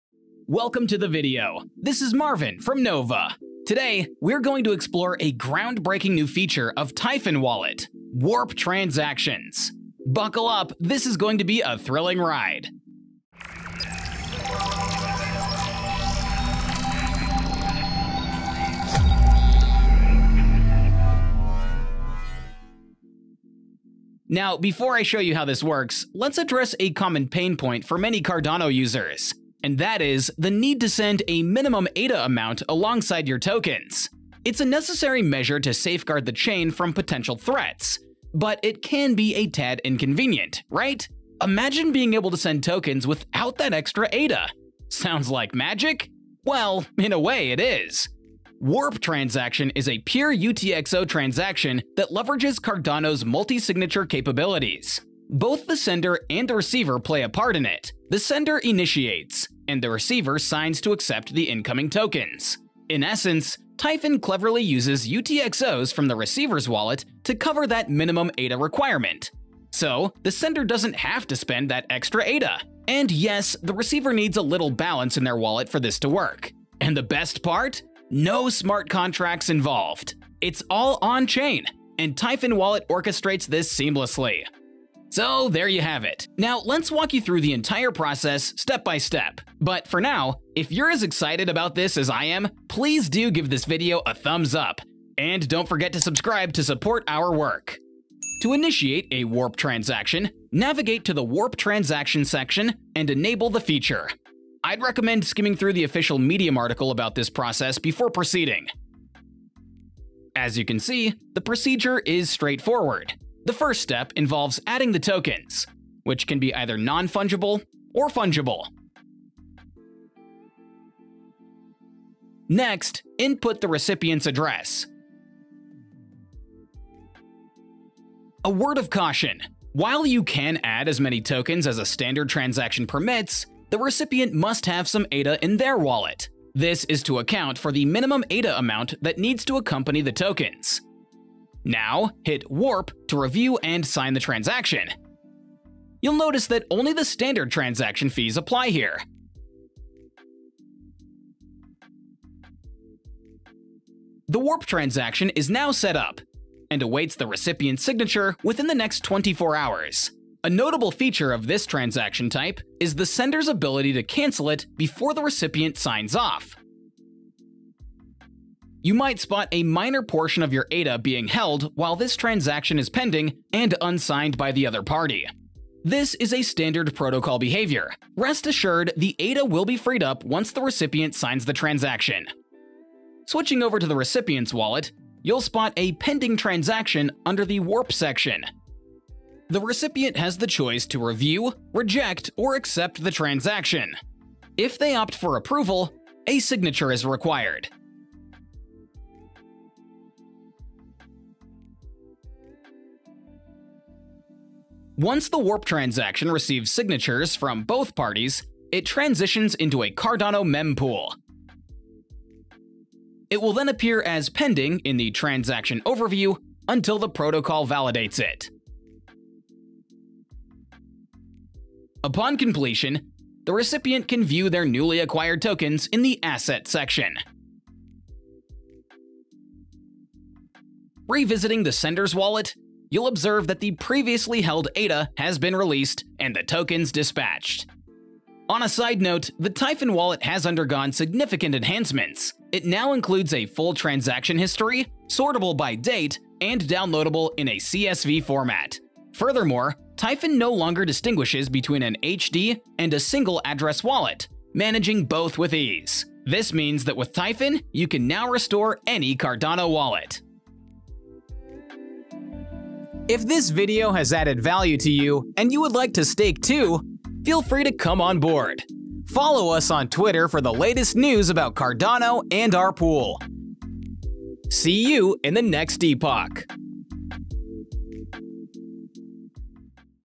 Typhon Light Wallet | Warp Transactions [English]
In this tutorial video we're exploring Typhon Wallet’s new feature: Warp Transactions! This multisig transaction is only possible because of a clever use of Cardano’s UTxO architecture and stands as a testament to innovation, offering a refined approach to token transfers. This video provides a methodical walkthrough, coupled with an overview of Typhon Wallet’s recent advancements.

𝗧𝗶𝗺𝗲𝘀𝘁𝗮𝗺𝗽𝘀:
0:00 Intro
0:24 What is a Warp Transaction?
1:25 Perform a Warp Transaction
3:58 Side Notes
4:21 Outro

May the rewards be with you!